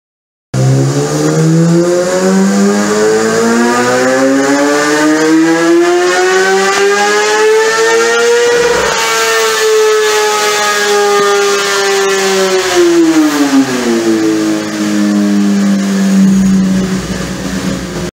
Dynojet PCIII zx6r 2003 PHP
Before and after configure Power Commander III USB with Dynojet to the only one and best of all Motorcycle garage in Cyprus PHP Bike Source.